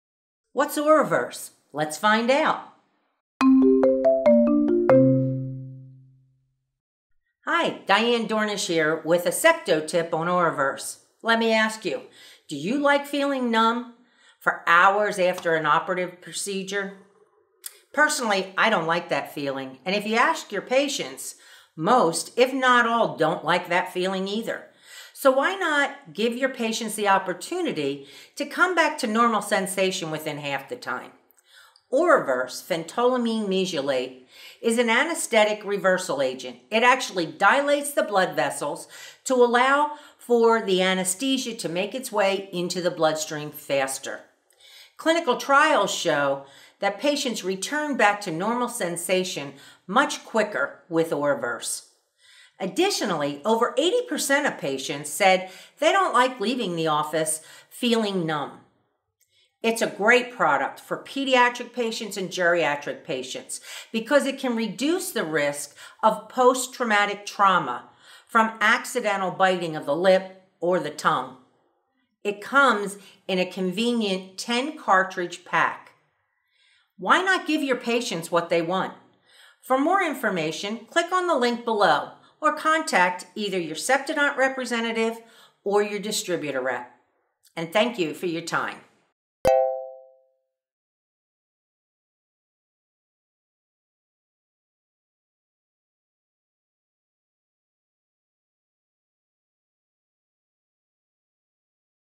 Septo-Tip: OraVerse
OraVerse (Phentolamine Mesylate) is the first and only local anesthesia reversal agent that allows you to bring patients back to normal sensation twice as fast. In clinical trials, the median time to recovery of normal sensation in the upper lip was 50 minutes for OraVerse patients vs. 133 minutes for the control group, and in the lower lip was 70 minutes for OraVerse patients vs. 155 minutes for the control.